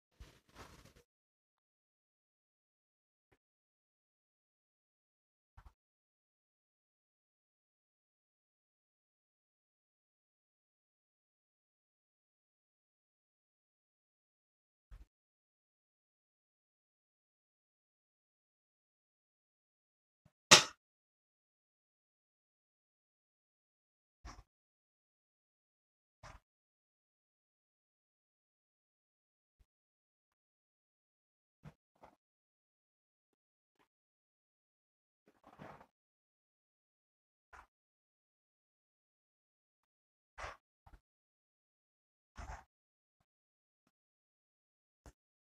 WHAT'S GOING ON(3)
Doing video of this and a very low flying helicopter went by